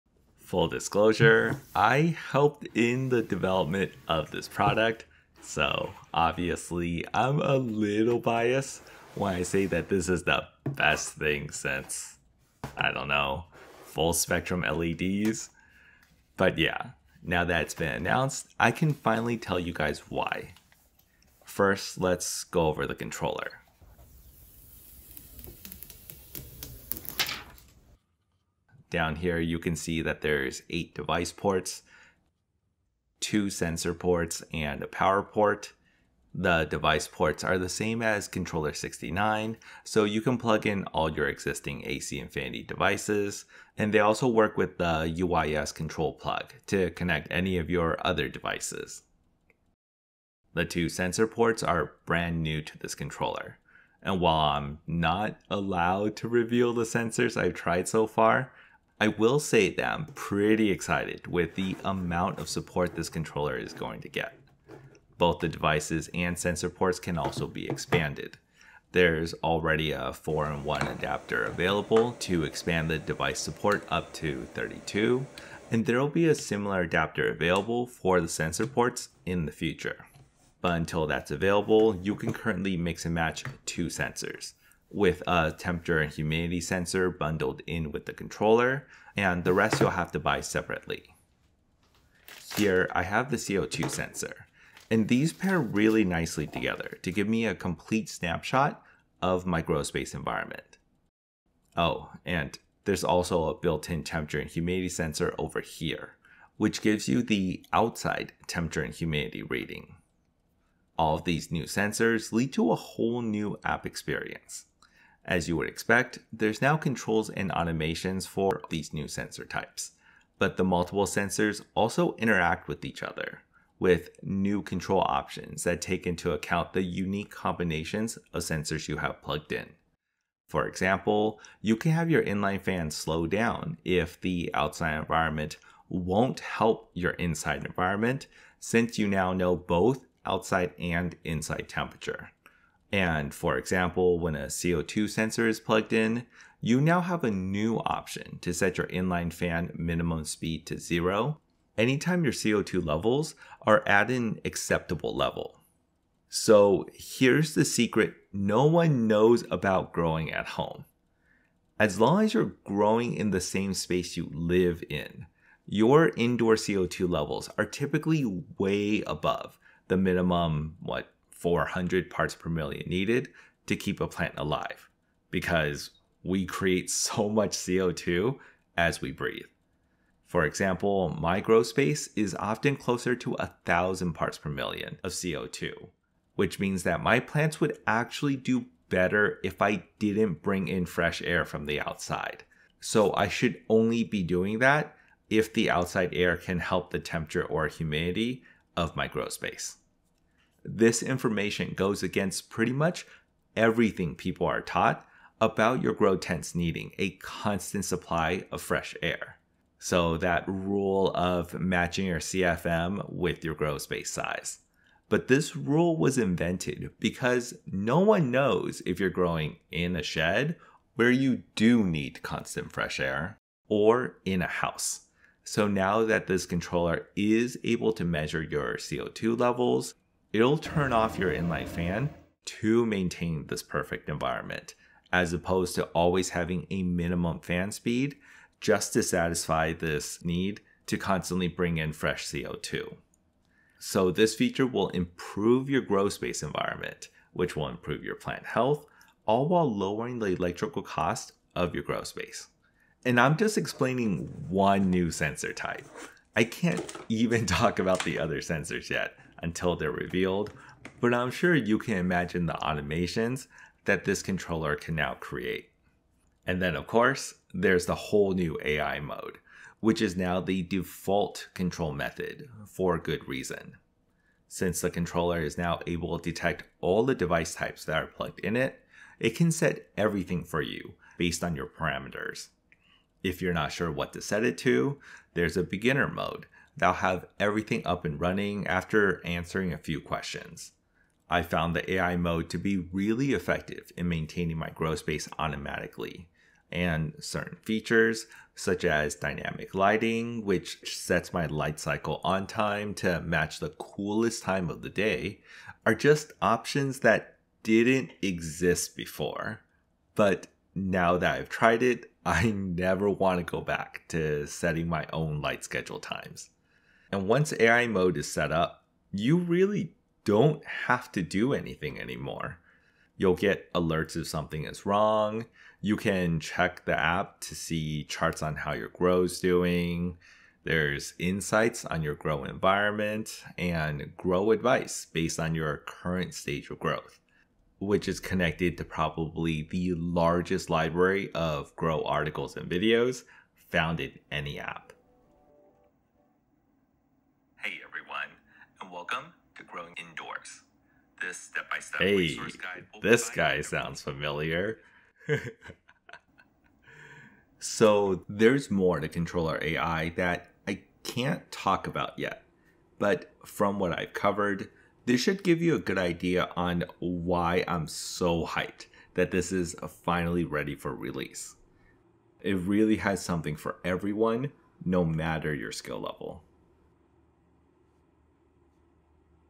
What I've Been Working On For the Past Year
Introducing the Controller AI from AC Infinity. I was brought in the help develop the features of this controller and to create the entire knowledge database for the app, which is mostly also posted on YouTube and can be found here:

Working on this project has been one of the best experiences of my life, and I'm really excited for all the pieces of this controller ecosystem to be released over the next few months.